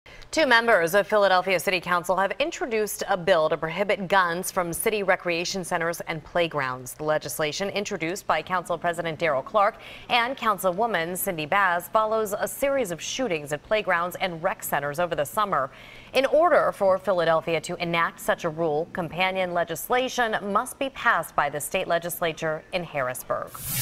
Bill Introduced To Prohibit Guns From Philadelphia Rec Centers, Playgrounds
2 members of Philadelphia City Council introduced the bill.